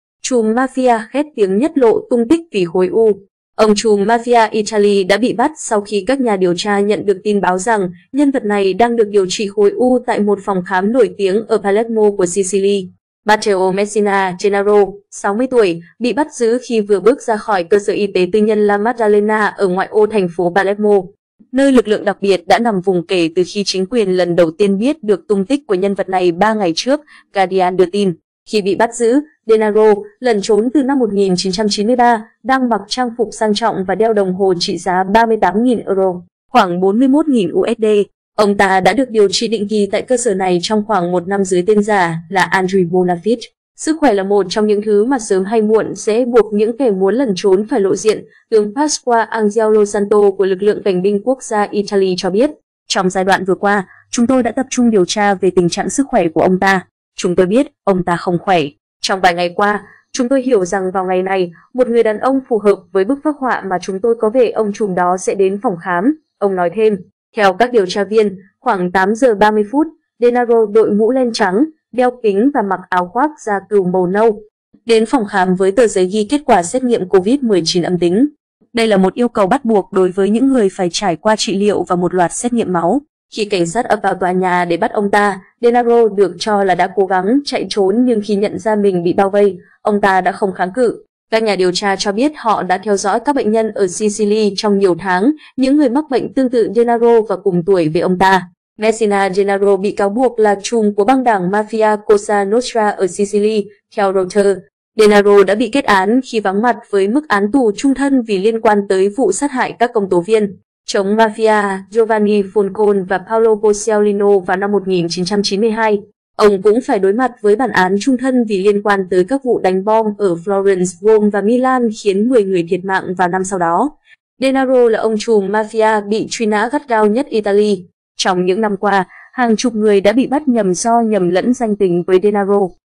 Trùm mafia khét tiếng nhất lộ tung tích vì khối u
Ông trùm mafia Italy đã bị bắt sau khi các nhà điều tra nhận được tin báo rằng nhân vật này đang được điều trị khối u tại một phòng khám nổi tiếng ở Palermo của Sicily.Khi cảnh sát ập vào tòa nhà để bắt ông ta, Denaro được cho là đã cố gắng chạy trốn nhưng khi nhận ra mình bị bao vây, ông ta đã không kháng cự.
Matteo Messina Denaro, 60 tuổi, bị bắt giữ khi vừa bước ra khỏi cơ sở y tế tư nhân La Maddalena ở ngoại ô thành phố Palermo, nơi lực lượng đặc biệt đã nằm vùng kể từ khi chính quyền lần đầu tiên biết được tung tích của nhân vật này 3 ngày trước, Guardian đưa tin.
Khi bị bắt giữ, Denaro - lẩn trốn từ năm 1993 - đang mặc trang phục sang trọng và đeo đồng hồ trị giá 38.000 euro (khoảng 41.000 USD).
 Matteo Messina Denaro bị cảnh sát chống mafia bắt ở Palermo. Ảnh: Cảnh binh quốc gia Italy.
Các nhà điều tra cho biết họ đã theo dõi các bệnh nhân ở Sicily trong nhiều tháng, những người mắc bệnh tương tự Denaro và cùng tuổi với ông ta.
Theo các điều tra viên, khoảng 8h30, Denaro đội mũ len trắng, đeo kính và mặc áo khoác da cừu màu nâu, đến phòng khám với tờ giấy ghi kết quả xét nghiệm Covid-19 âm tính. Đây là một yêu cầu bắt buộc đối với những người phải trải qua trị liệu và một loạt xét nghiệm máu.
Ông cũng phải đối mặt với bản án chung thân vì liên quan tới các vụ đánh bom ở Florence, Rome và Milan khiến 10 người thiệt mạng vào năm sau đó.
“Trong giai đoạn vừa qua, chúng tôi đã tập trung điều tra về tình trạng sức khỏe của ông ta. Chúng tôi biết ông ta không khỏe. Trong vài ngày qua, chúng tôi hiểu rằng vào ngày này, một người đàn ông phù hợp với bức phác họa mà chúng tôi có về ông trùm đó sẽ đến phòng khám”, ông nói thêm.
Denaro là ông trùm mafia bị truy nã gắt gao nhất Italy. Trong những năm qua, hàng chục người đã bị bắt nhầm do nhầm lẫn danh tính với Denaro.
Messina Denaro bị cáo buộc là trùm của băng đảng mafia Cosa Nostra ở Sicily, theo Reuters.
Ông ta đã được điều trị định kỳ tại cơ sở này trong khoảng một năm dưới tên giả là Andrea Bonafede. “Sức khỏe là một trong những thứ mà sớm hay muộn sẽ buộc những kẻ muốn lẩn trốn phải lộ diện”, tướng Pasquale Angelosanto của lực lượng cảnh binh quốc gia Italy cho biết.
Denaro đã bị kết án khi vắng mặt với mức án tù chung thân vì liên quan tới vụ sát hại các công tố viên chống mafia Giovanni Falcone và Paolo Borsellino vào năm 1992.